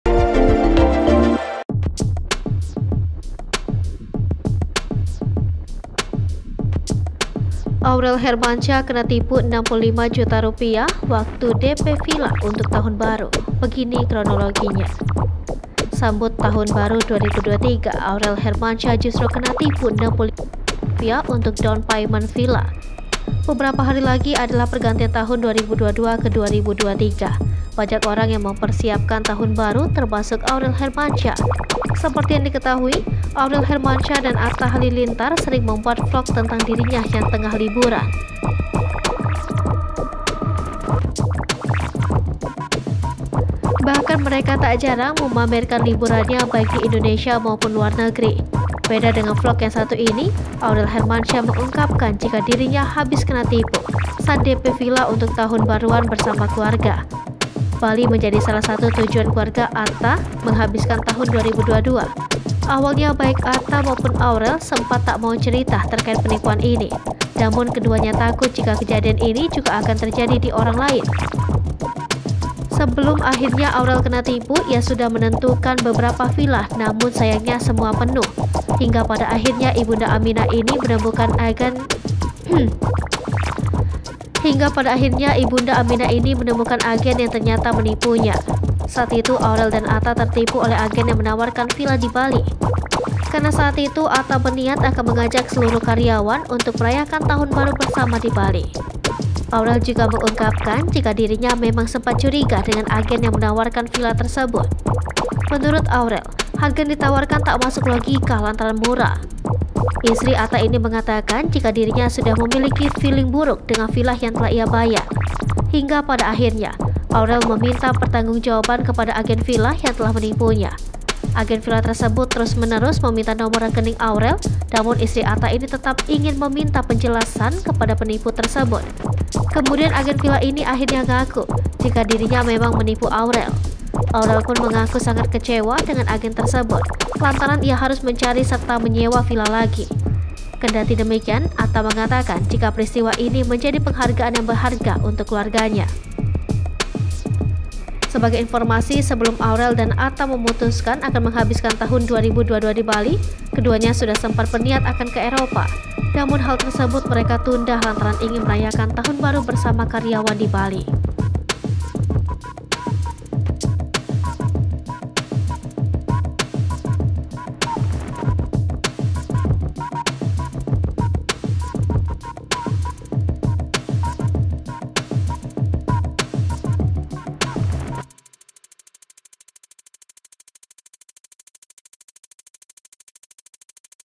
Kronologi Aurel Hermansyah Kena Tipu Rp 65 Juta Waktu DP Villa Untuk Tahun Baru
Sambut tahun baru 2023, Aurel Hermansyah justru kena tipu Rp 65 juta untuk Down Payment (DP) villa.

Beberapa hari lagi adalah pergantian tahun 2022 ke 2023, banyak orang yang mempersiapkan tahun baru termasuk Aurel Hermansyah.

Seperti yang diketahui, Aurel Hermansyah dan Atta Halilintar sering membuat vlog tentang dirinya yang tengah liburan.

Lihat informasi terkini bersama kami, di:

Palembang.tribunnews.com

Sumber Video:
Editor Video: